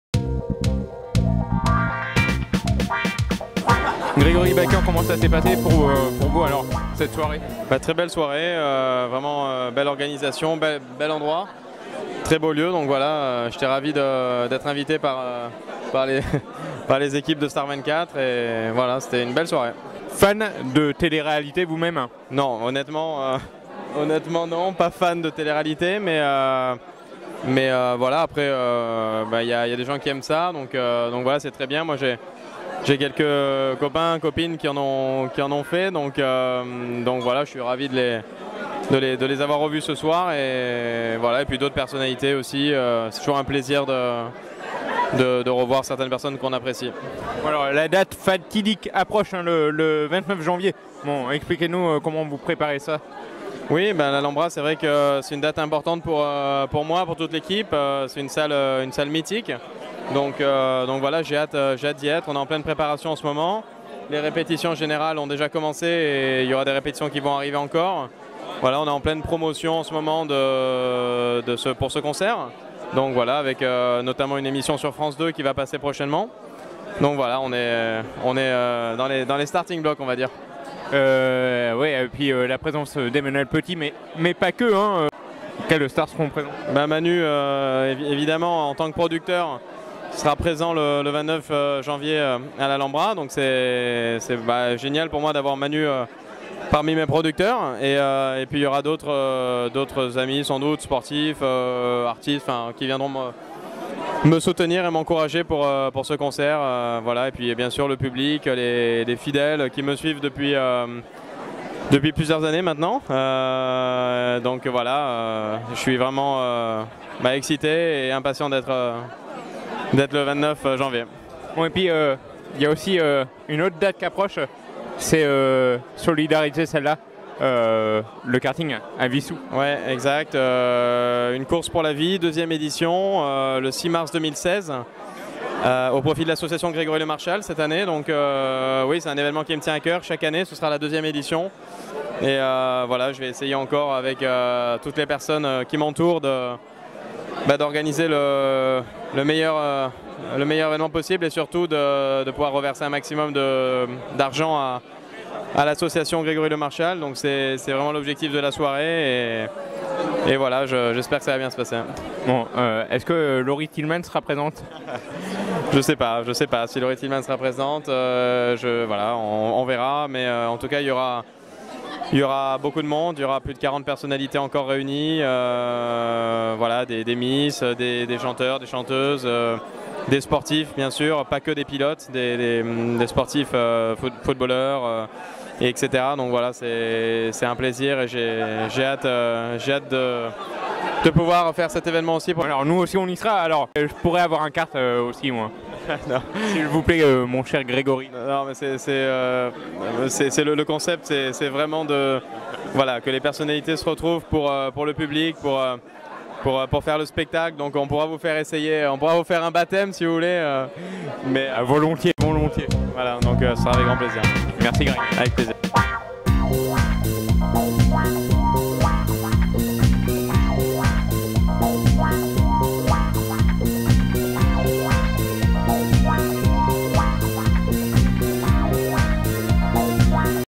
Grégory Bakian - Lauriers TV Awards 2016
Grégory Bakian invité et spectateur à la cérémonie des Lauriers TV Awards 2016.